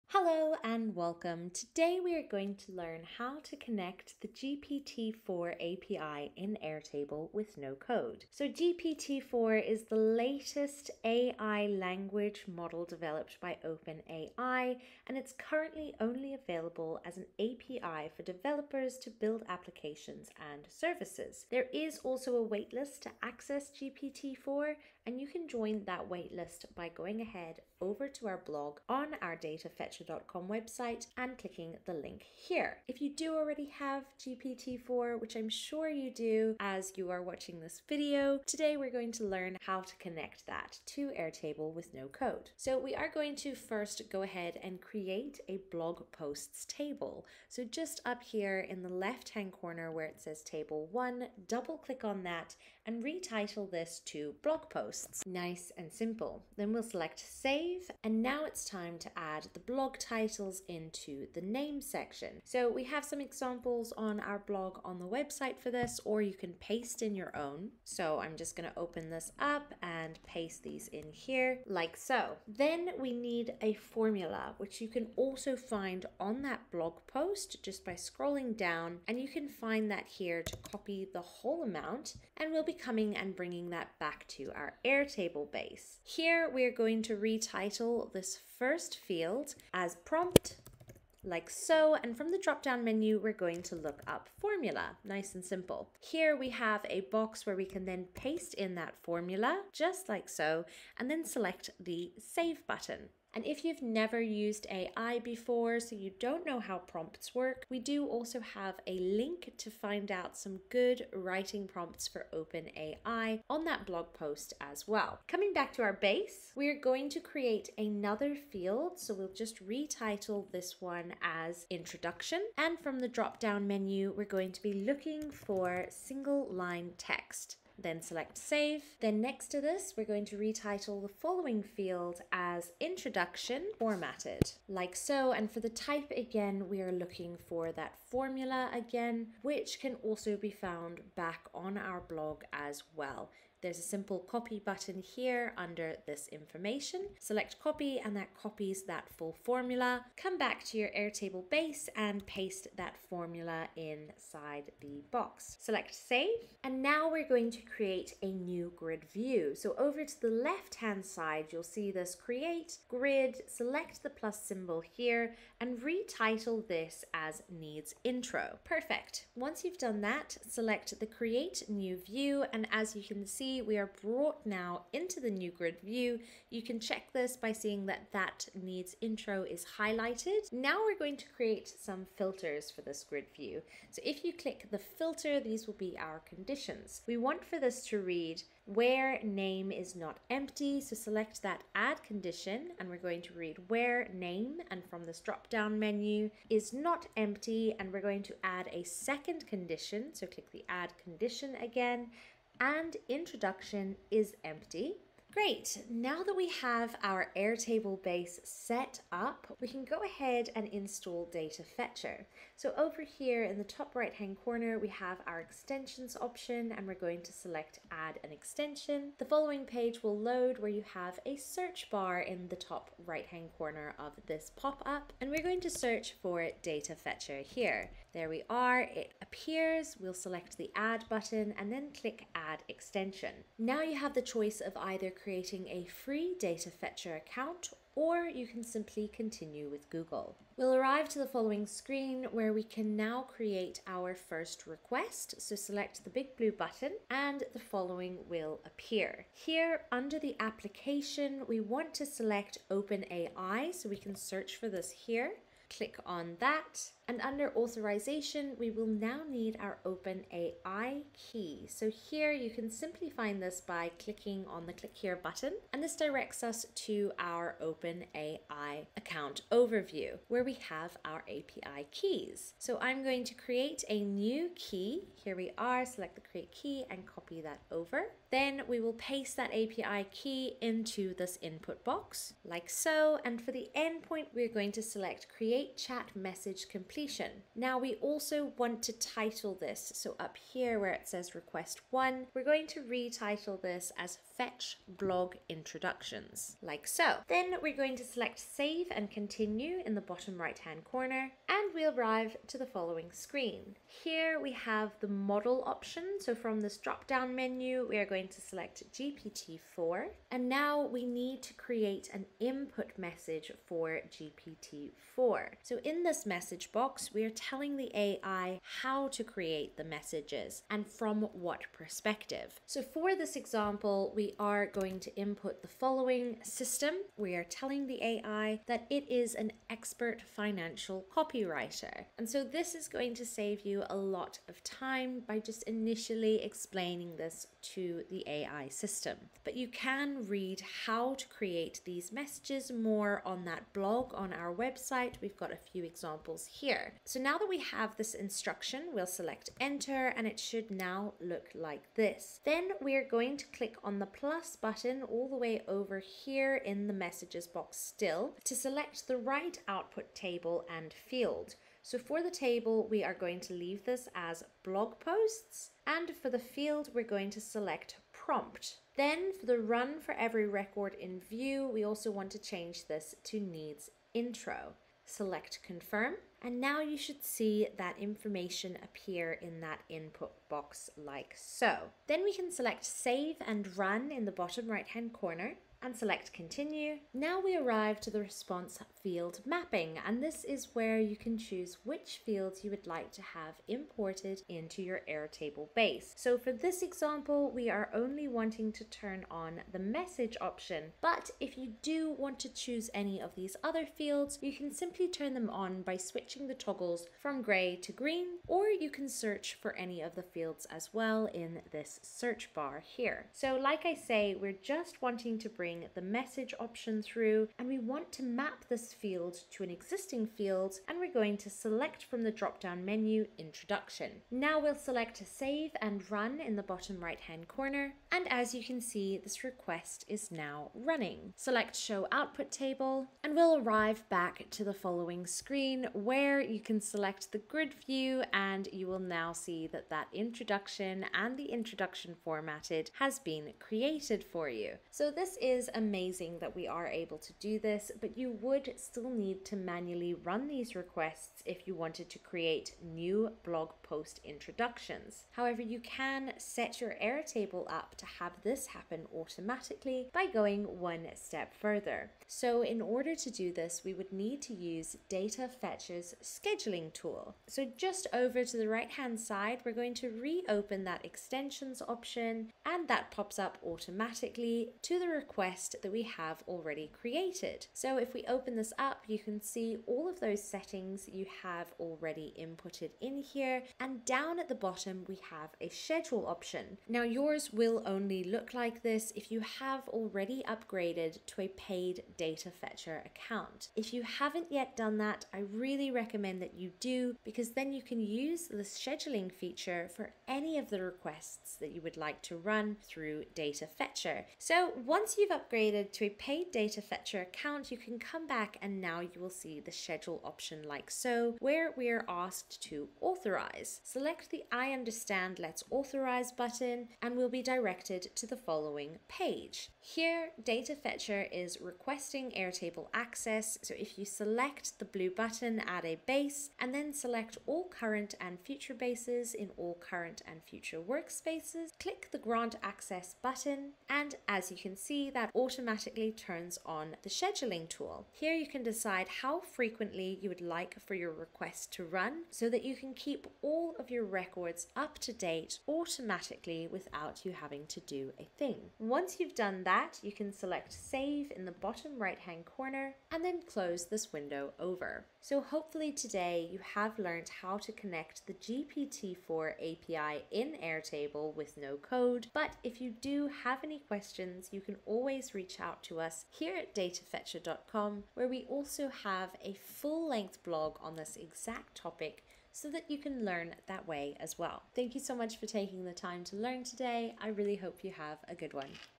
Connect to the GPT-4 API in Airtable with No-Code
In this guide, we'll connect the GPT-4 API to Airtable and use this to create a series of introduction paragraphs for various financial blog posts.

The GPT-4 API is now generally available without the waitlist mentioned in the video. You will need to have a payment card set up for the OpenAI API. This is a different thing from ChatGPT Plus.

We'll be using the OpenAI Airtable integration and Data Fetcher. This requires no code and can all be done for free.

0:00: Intro
0:42: Set up "Blog posts" table
3:29: Add Data Fetcher extension
4:00: Set up OpenAI request
6:22: Map fields & import data
7:27: Schedule request to run automatically